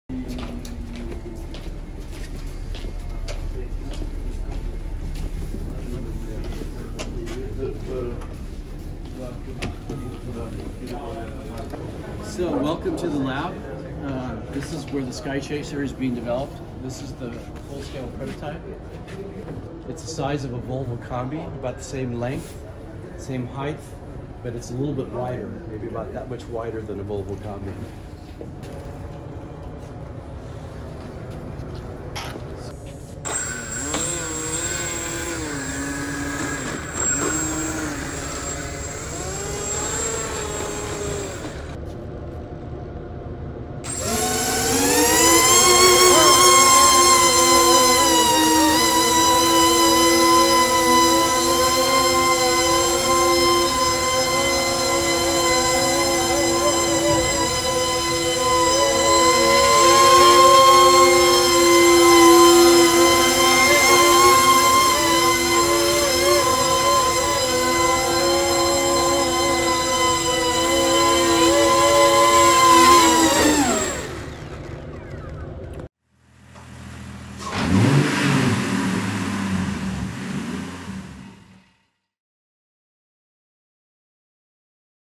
Flygbilen Sky Chaser flyger (i alla fall en modell)
Flygande bilar? Finns det verkligen? Ja, i Fjugesta. I alla fall ett par prototyper. Kom ihåg var du såg Sky Chaser för första gången!